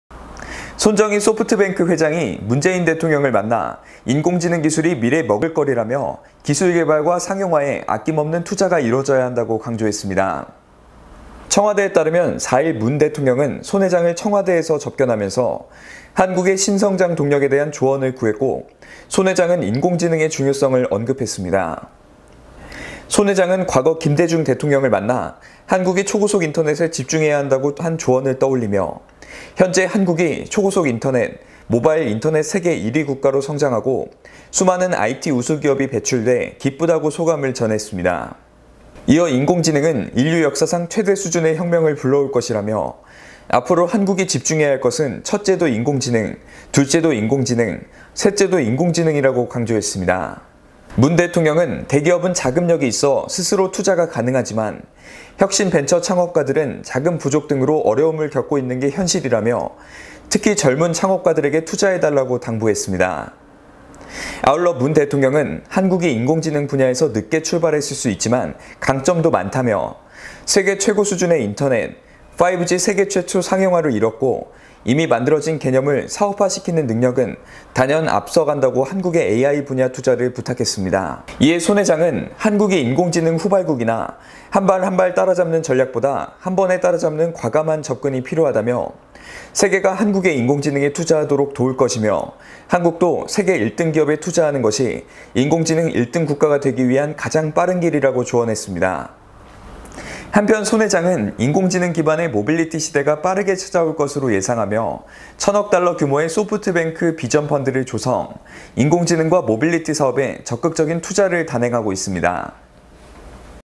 [라이브썰] 손정의 소프트뱅크 회장 "인공지능 기술이 미래 먹을거리"
손정의 소프트뱅크 회장이 문재인 대통령을 만나 ‘인공지능’(AI) 기술이 미래 먹을거리라며 기술 개발과 상용화에 아낌없는 투자가 이뤄져야한다고 강조했습니다.

청와대에 따르면 4일 문 대통령은 손 회장을 청와대에 접견하면서 한국의 신성장동력에 대한 조언을 구했고 손 회장은 인공지능의 중요성을 언급했습니다.

손 회장은 과거 김대중 대통령을 만나 한국이 초고속 인터넷에 집중해야 한다고 한 조언을 떠올리며 “현재 한국이 초고속 인터넷, 모바일 인터넷 세계1위 국가로 성장하고 수많은 IT우수 기업이 배출돼 기쁘다”고 소감을 전했습니다.

이어 “인공지능은 인류역사상 최대 수준의 혁명을 불러올 것”이라며 “앞으로 한국이 집중해야 할 것은 첫째도 인공지능, 둘째도 인공지능, 셋째도 인공지능”이라고 강조했습니다.

문 대통령은 “대기업은 자금력이 있어 스스로 투자가 가능하지만 혁신벤처창업가들은 자금부족 등으로 어려움을 겪고 있는 게 현실”이라며 “특히 젊은 창업가들에게 투자해 달라”고 당부했습니다.

아울러 문 대통령은 “한국이 인공지능 분야에서 늦게 출발했을 수 있지만 강점도 많다”며 “세계 최고 수준의 인터넷, 5G 세계 최초 상용화를 이뤘고 이미 만들어진 개념을 사업화시키는 능력은 단연 앞서 간다”고 한국의 AI 분야 투자를 부탁했습니다.

이에 손 회장은 “한국이 인공지능 후발국이나 한발 한발 따라잡는 전략보다 한 번에 따라잡는 과감한 접근이 필요하다”며 “세계가 한국의 인공지능에 투자하도록 도울 것이며 한국도 세계 1등 기업에 투자하는 것이 인공지능 1등 국가가 되기 위한 가장 빠른 길”이라고 조언했습니다.

한편 손 회장은 인공지능 기반의 모빌리티 시대가 빠르게 찾아올 것으로 예상하며 1000억 달러 규모의 소프트뱅크비전펀드를 조성, 인공지능과 모빌리티 사업에 적극적인 투자를 단행하고 있습니다.